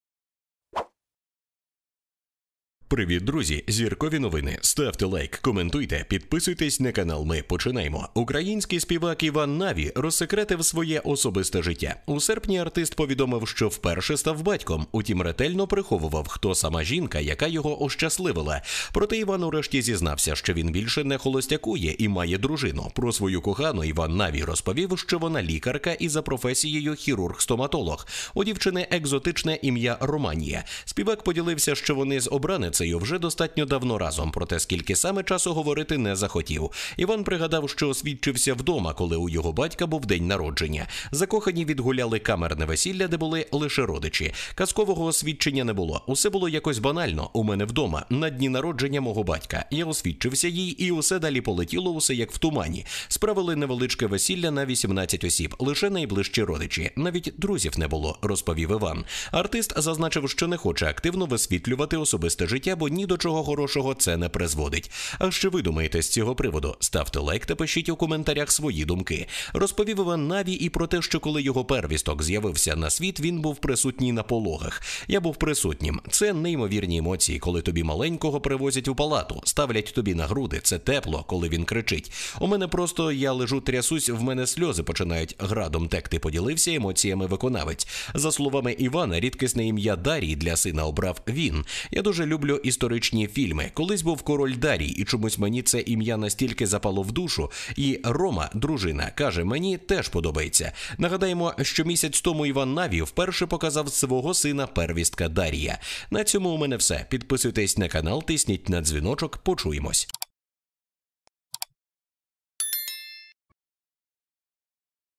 Приховував до останнього! NAVI зізнався вперше - кільце на пальці і дитина: ОЦЕ ТАК. Фани в шоці!
Український співак Ivan NAVI розсекретив своє особисте життя.